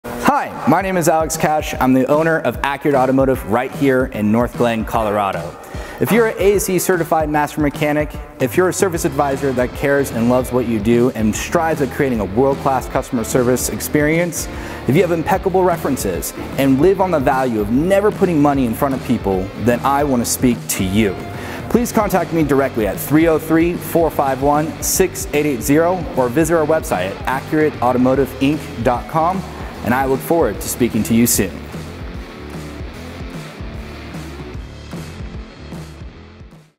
Accurate Automotive Introduction Video
At Accurate Automotive we service all makes and models and no job is to big or small for our shop.

We are growing and looking for someone who has pride, passion, and lives on the value of never putting money in front of people to join our team!